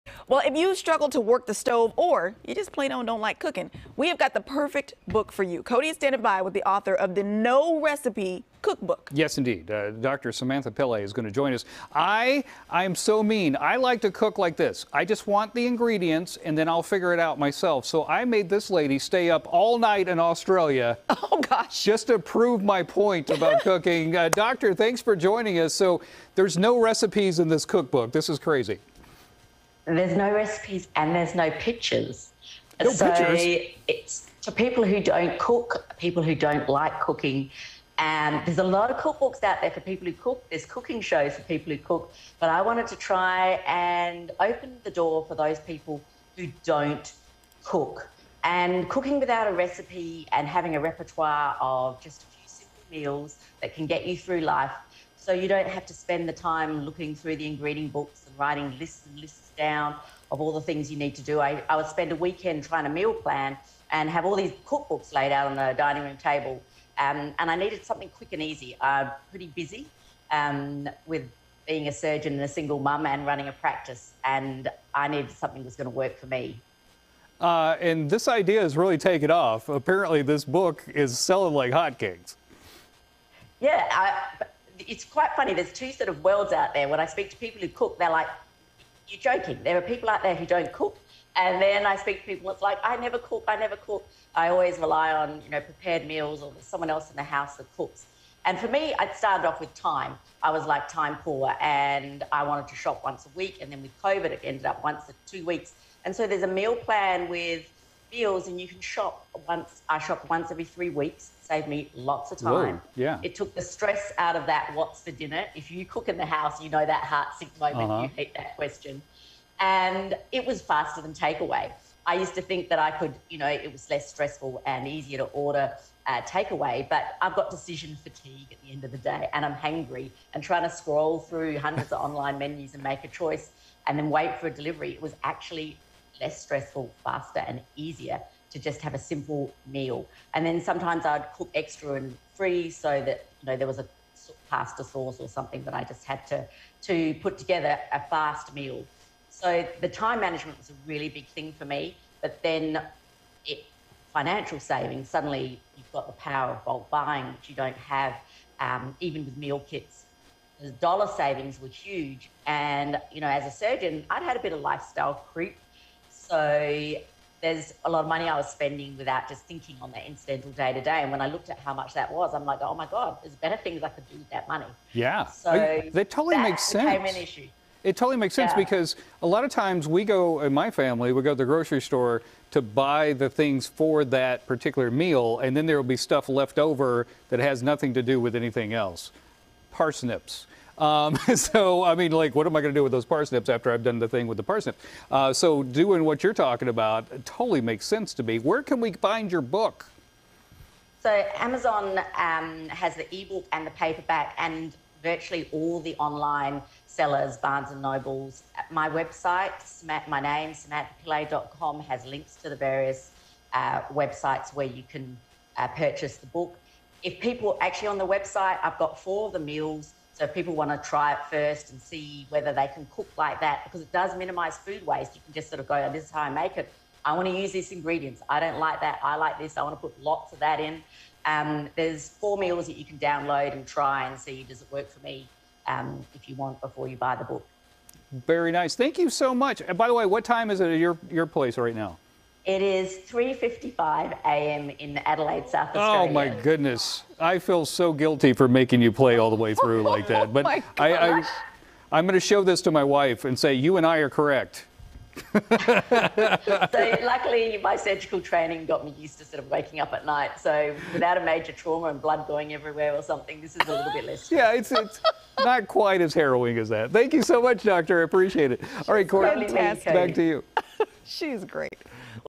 The No Recipe Cookbook
Cody talks to the author of "The No Recipe Cookbook"